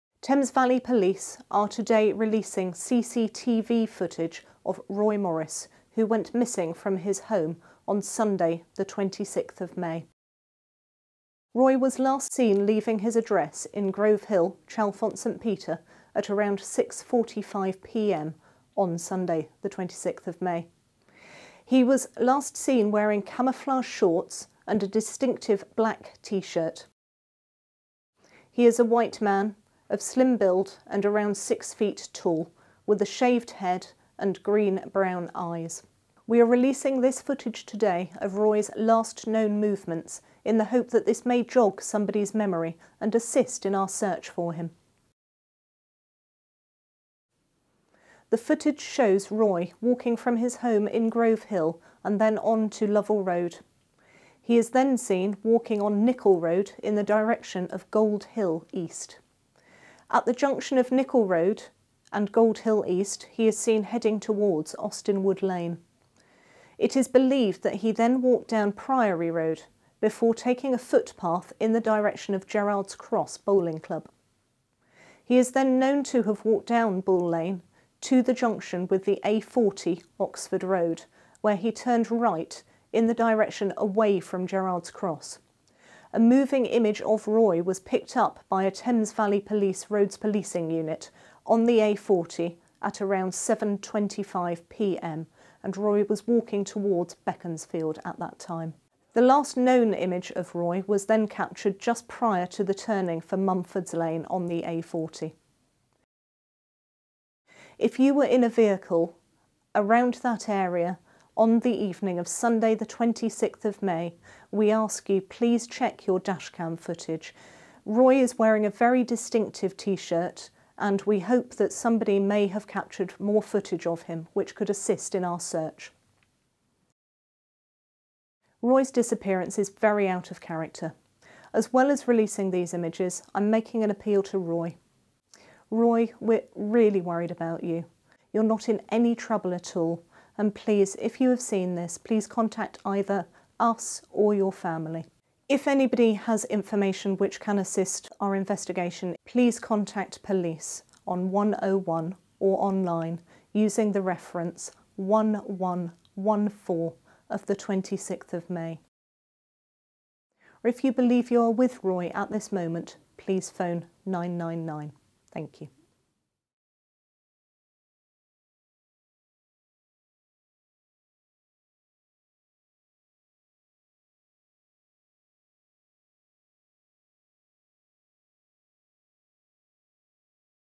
CCTV footage released in search for Roy Morris.
We are releasing CCTV footage of missing Chalfton St Peter man Roy Morris aged 60.